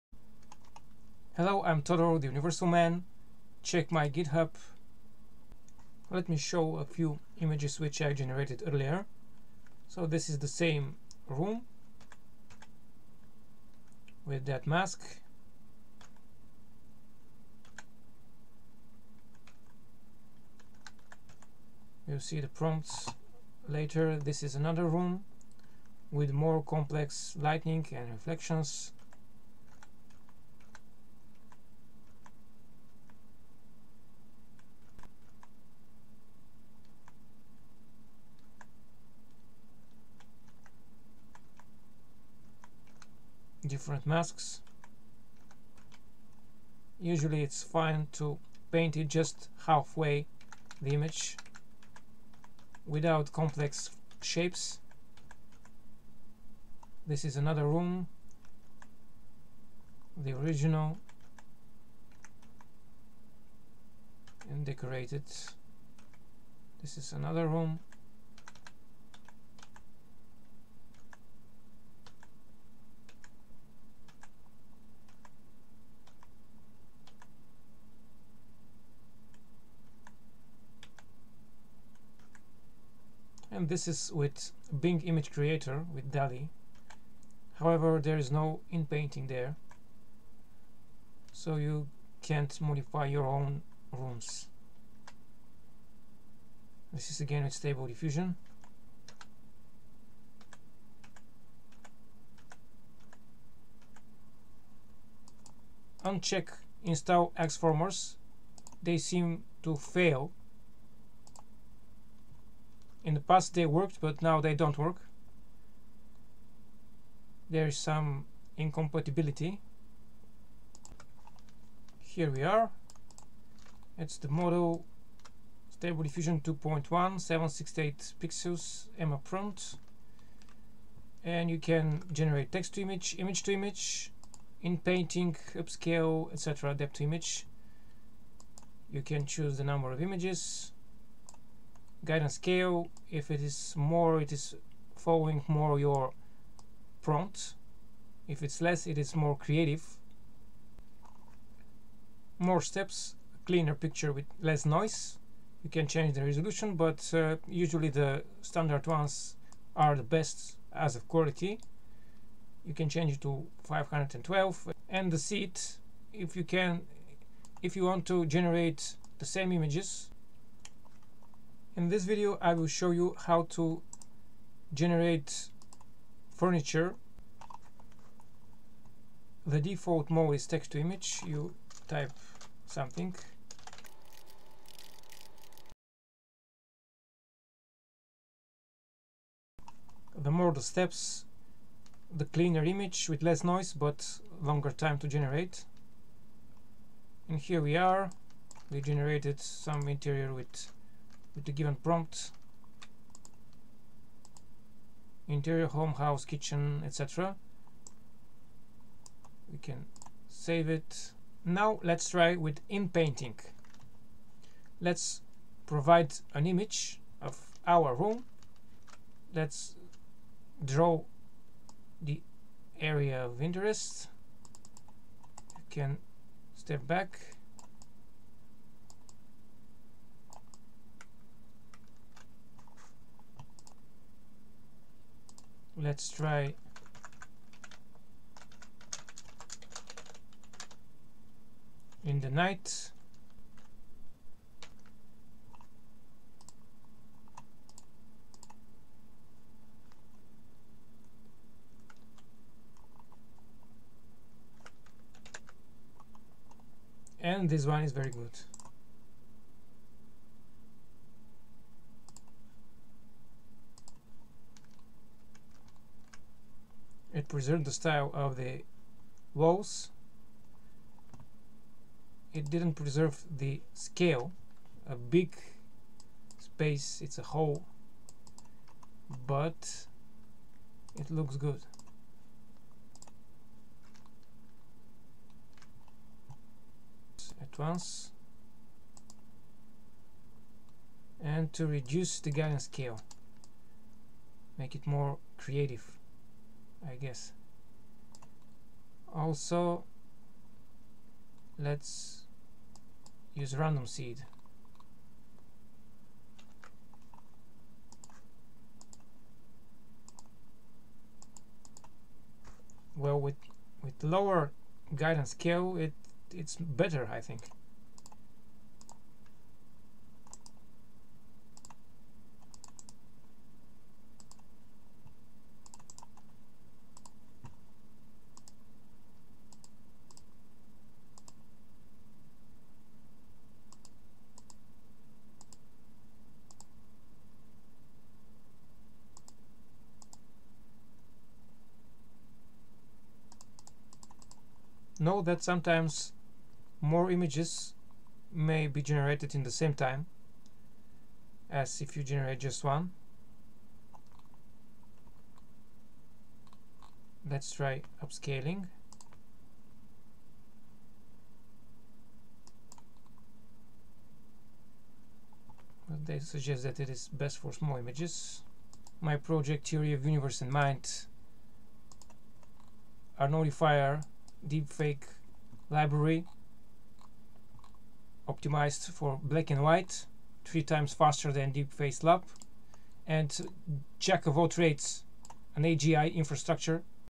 Stable Diffusion for Interior Design and Furniture Inpainting Tutorial Tips with Google Colab
An introduction to generating furniture/room images with Stable Diffusion inpainting models. Simple prompt-engineering.
Check the other videos for Stable Diffusion, GPT2 and other generative techniques in the channel and in my github.